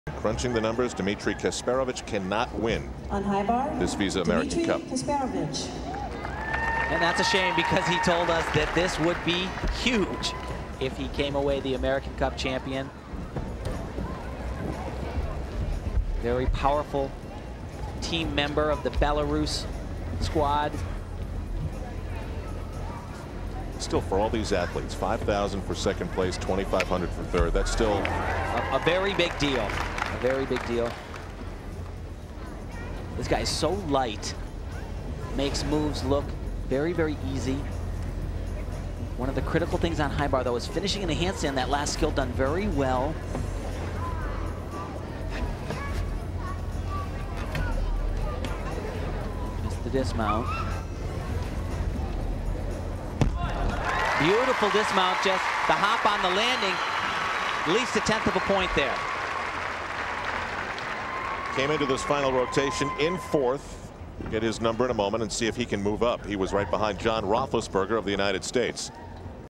Dmitri Kasperovitch - High Bar - 2000 Visa American Cup
Score: 9.325. Dmitri Kasperovitch (BLR)
Feb. 26, 2000 - Orlando Arena - Orlando, Fla.